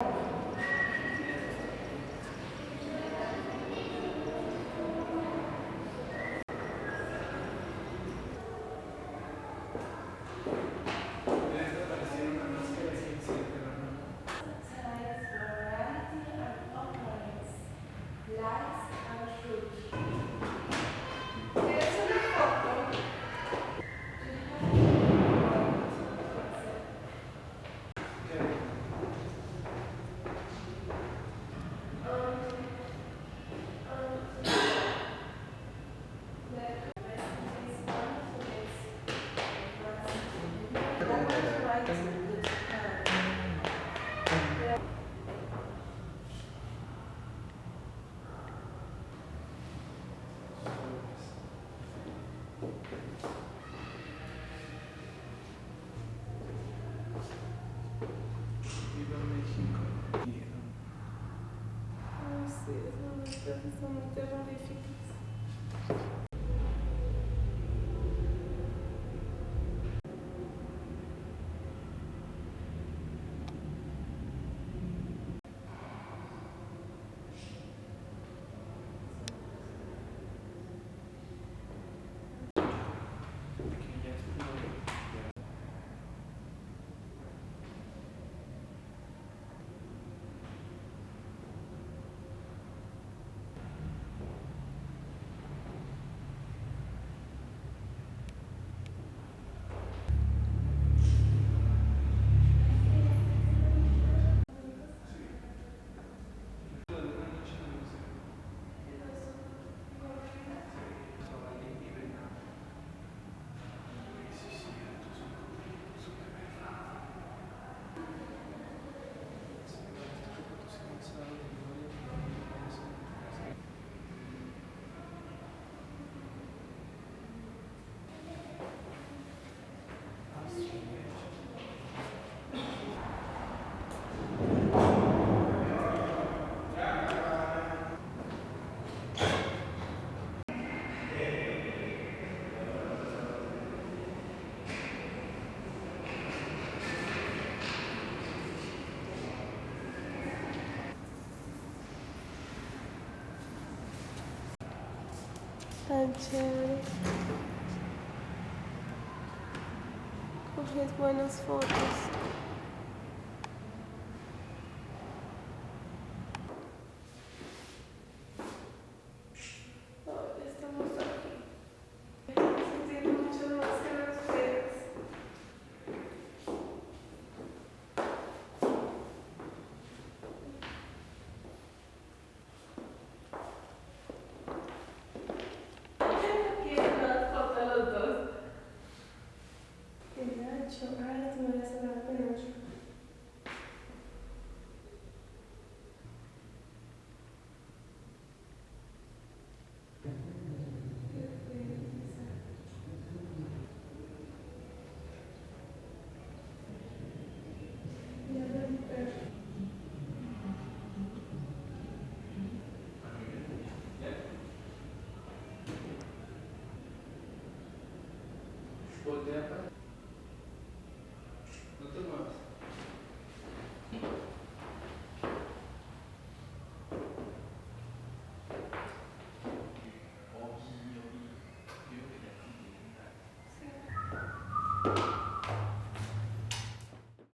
Museo Nacional de la Mascara, San Luis Potosi, Mexico
El Museo Nacional de la Máscara es un museo ubicado la ciudad mexicana de San Luis Potosí, que está dedicado a proteger y promover el patrimonio ritual de la danza con máscaras de México desde el periodo prehispánico hasta el presente. Se encuentra en una antigua mansión en la Plaza del Carmen, que se convirtió en propiedad federal en 1907 y aloja al museo desde 1982. La colección permanente contiene aproximadamente 1300 piezas, la más grande de México, y está compuesta casi en su totalidad máscaras y trajes de danza mexicanos.